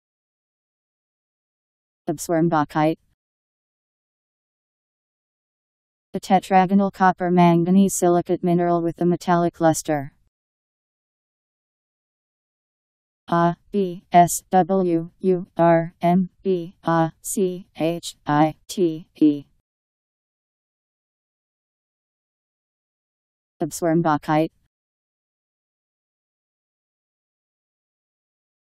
Abswurmbachite Meaning
Video shows what abswurmbachite means. A tetragonal copper manganese silicate mineral with a metallic lustre..  Abswurmbachite Meaning. How to pronounce, definition audio dictionary. How to say abswurmbachite. Powered by MaryTTS, Wiktionary